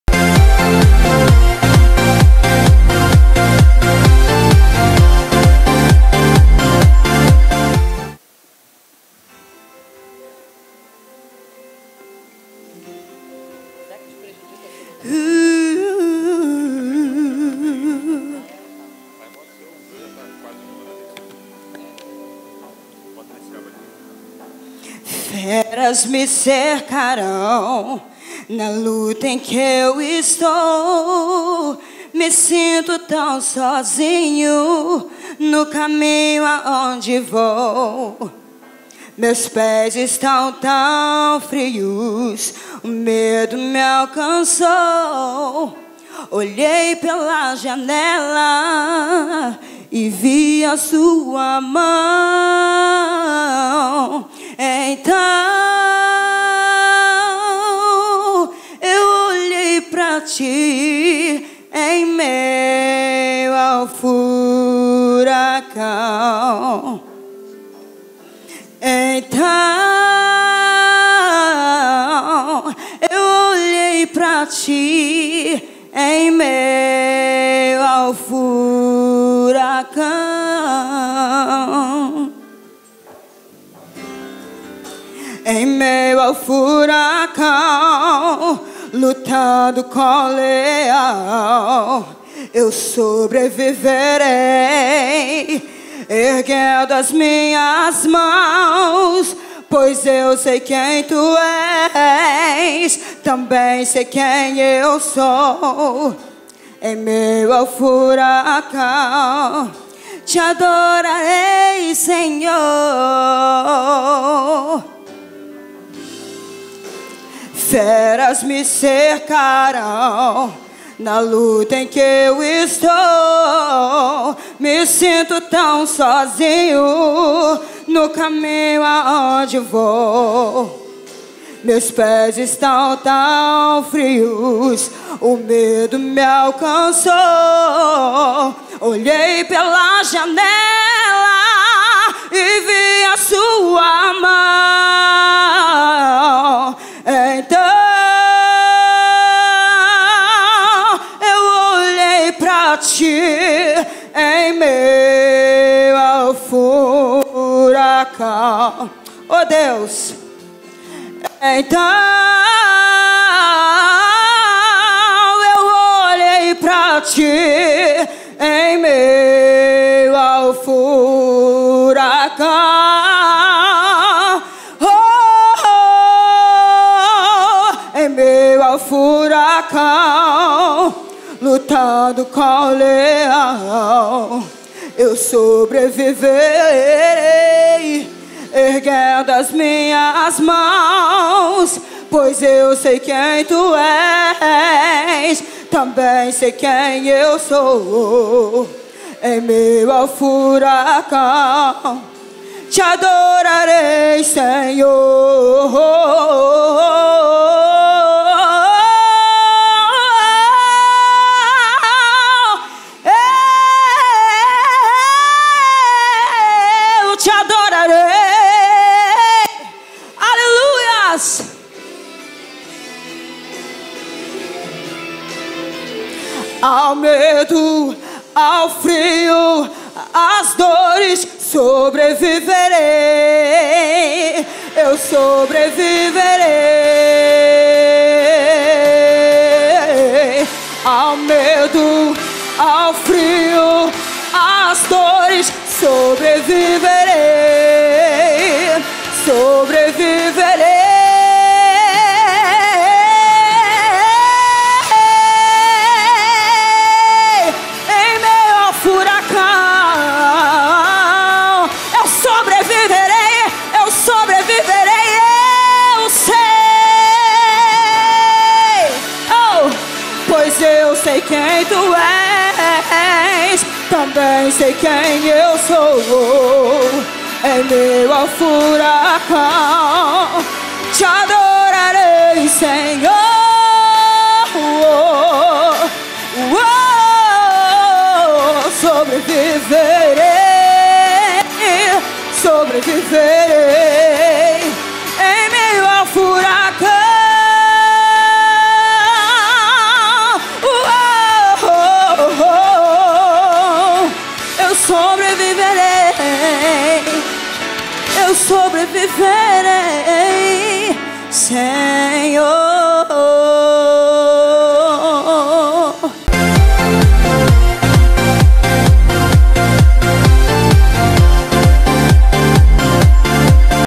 Finalista Ana Raquel [FESTIVAL CANTA CELEBRAI]
Confira participação da finalista do Festival Canta Celebrai Ana Raquel com sua interpretação belíssima.
A Candidata foi analisada em 4 critérios por uma banca de jurados especializada no ramo musical. Em março teremos nova edição do Festival Canta Celebrai. Um projeto da Gravadora Celebrai Music e produção da agência Gênesis Primeiro design.